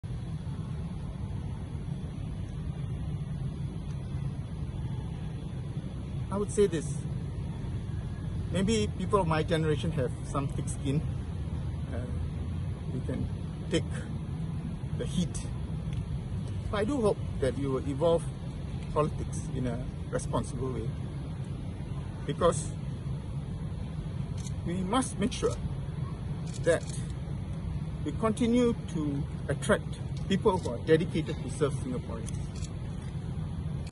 PAP's Murali Pillai talks about a post that was seen as a “scurrilous attack” on his family
The People's Action Party's (PAP) Bukit Batok SMC candidate Murali Pillai was emotional when asked about an online post that he had described as a "scurrilous attack" on his family. Mr Murali, 57, spoke to the media on Wednesday morning (July 1) at a walkabout in the constituency.

ST VIDEO: ZAIHAN MOHAMED YUSOF